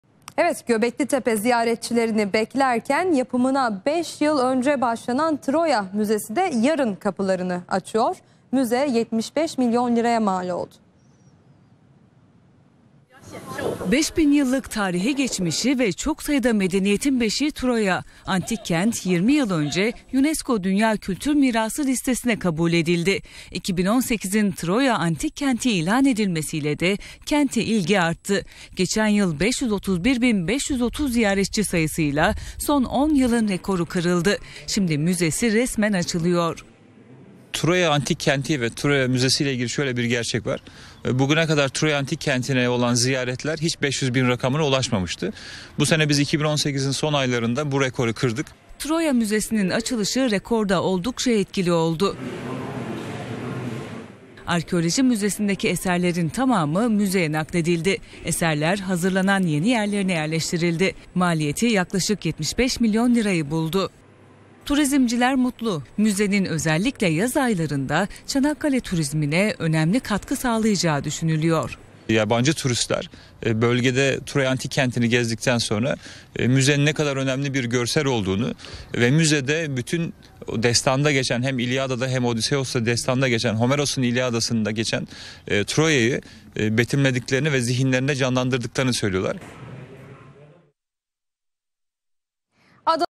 Ahmet Çelik - Haber Global
Birliğimiz Çanakkale - Körfez BTK Başkanı Ahmet Çelik, Haber Global' de yayınlanan röportajında, Troya Müzesi açılışının turizm sektörüne katkıları hakkında açıklamalarda bulundu.